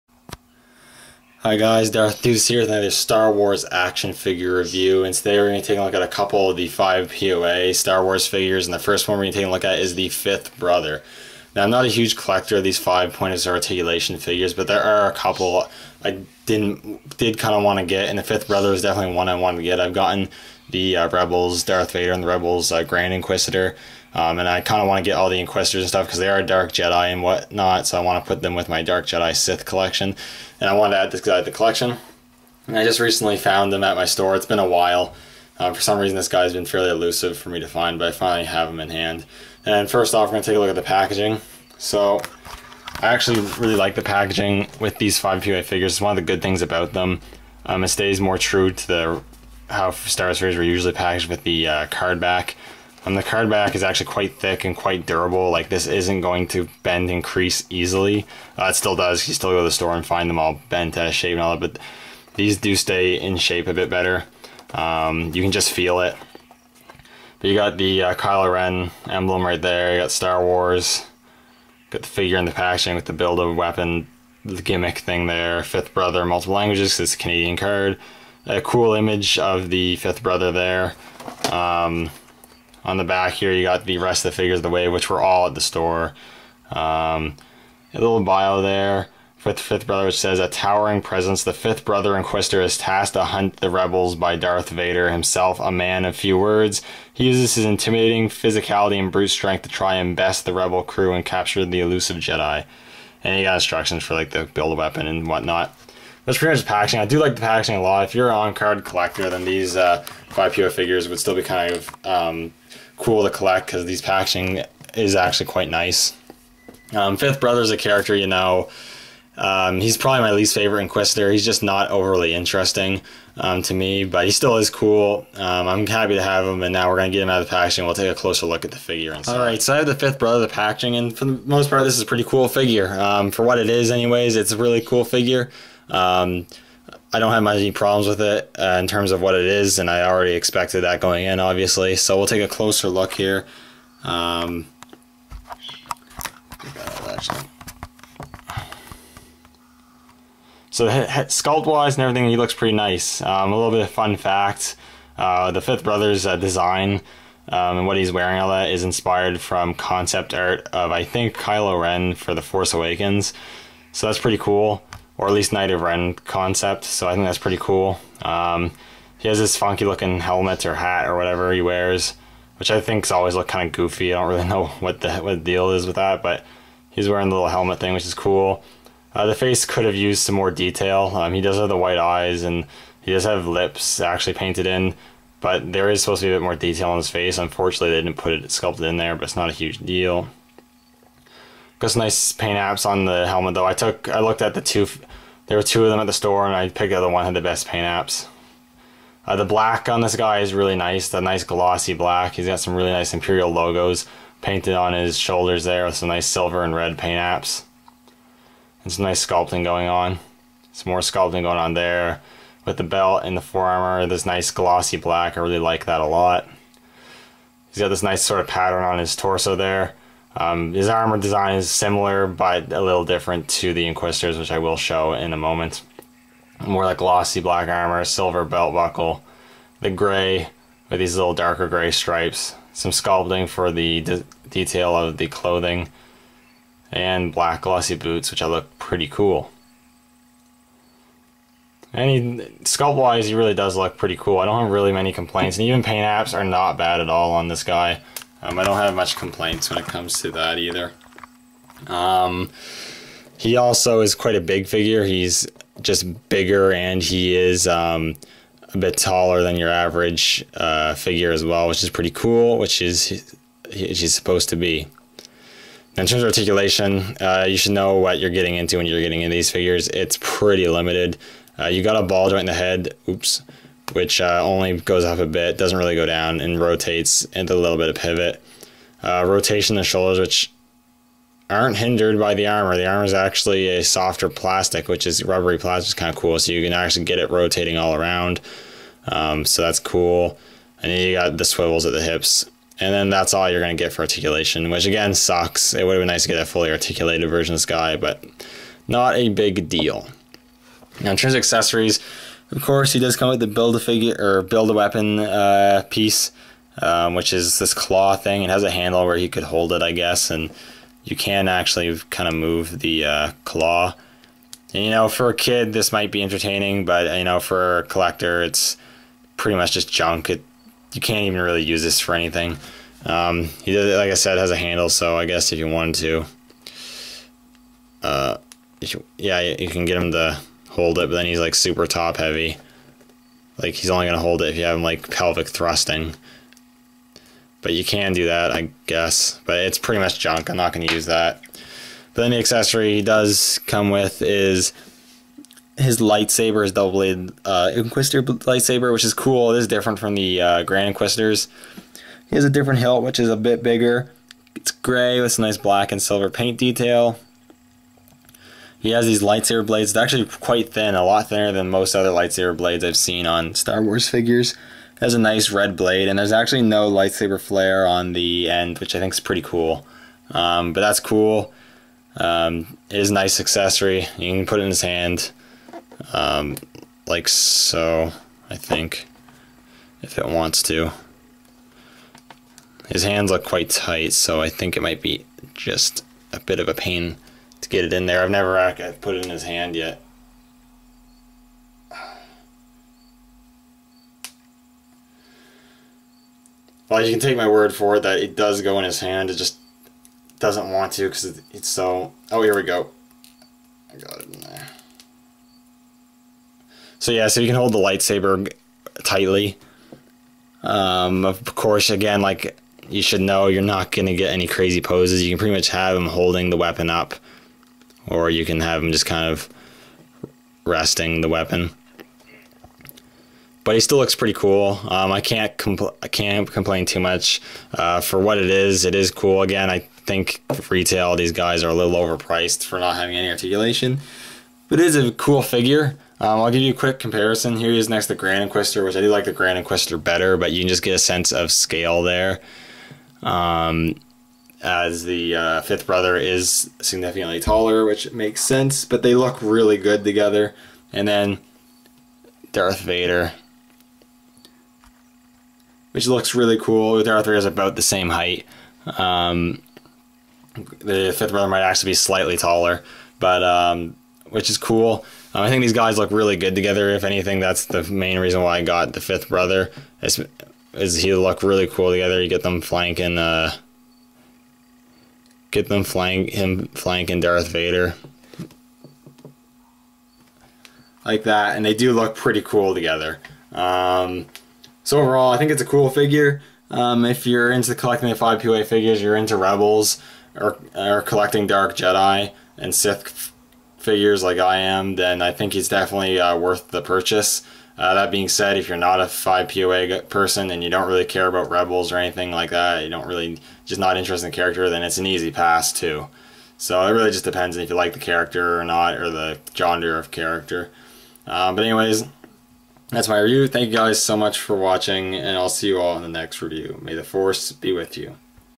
Fifth Brother - Star Wars Rebels Figure Review
A review on the 5POA Star Wars Rebels Fifth Brother Inquisitor figure!